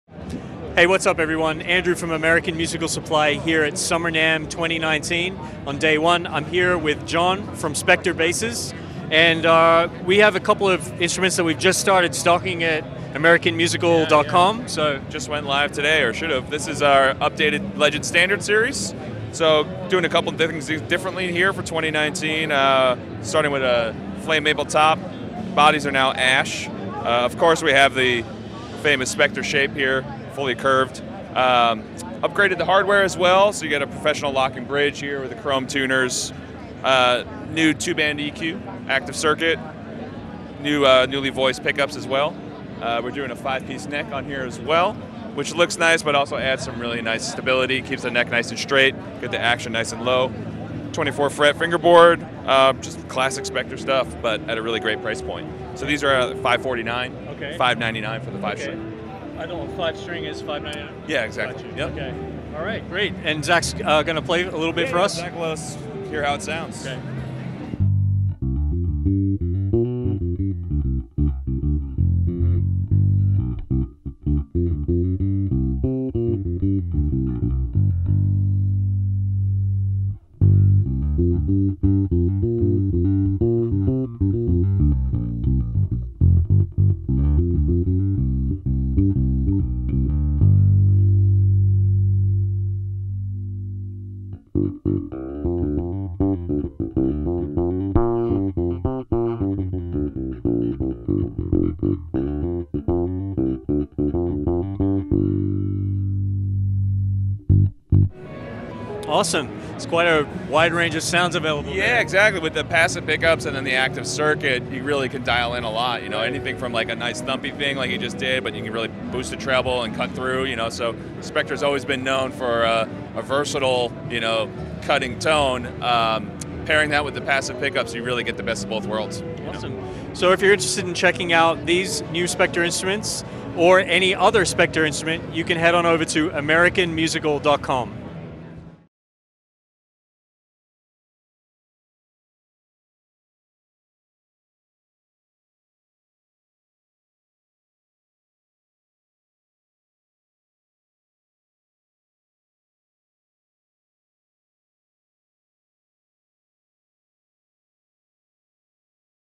Summer NAMM 2019 - Spector Basses - American Musical Supply
The instantly familiar, curved body design of the Legend 5 Standard is now crafted from solid, lightweight ash and adorned with a flame maple top. Other key improvements include a 5-piece maple/padauk neck, all-new pickups and electronics, improved hi-mass bridge, and all new gloss finishes!

Spector Legend 5 Standard 5-String Bass Guitar Features:

- Carved ash body with a genuine flame maple top
- Five-piece maple and padauk neck with a 24 fret Amara fingerboard
- Specially designed Spector humbucker pickup set
- Two-band active EQ controls
- Professional quality chrome hardware and hi-mass bridge

The Legend 5 Standard offers the coveted Spector NS body design at a price that is well within the reach of all bassists. The extremely comfortable curved body is crafted from solid ash and sports a genuine flame maple top, five-piece maple and padauk neck, a 24 fret Amara fingerboard, professional-quality chrome hardware, and a hi-mass bridge. Specially designed Spector humbucker pickups are complemented by the two-band active EQ controls that offer 12dB of boost or cut for both treble and bass controls. Adorned with a high gloss finish, the Legend 5 Standard offers the looks, tone, and playability of basses twice its price.